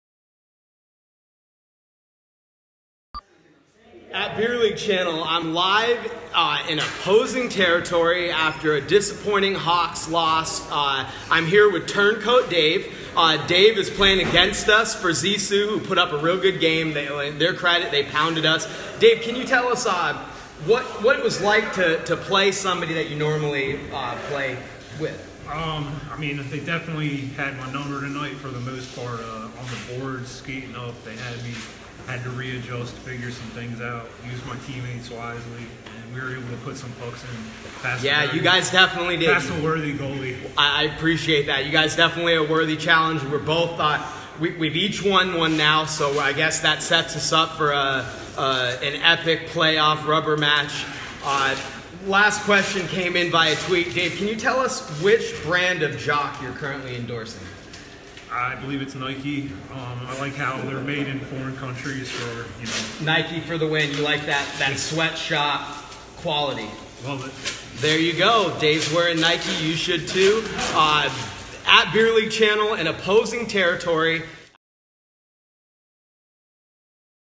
Posties! #9 Behind Enemy Lines. Post game hockey interview
Posties! #9 Behind Enemy Lines. PART 1: Post game interview with teammates and today’s opponent Dave.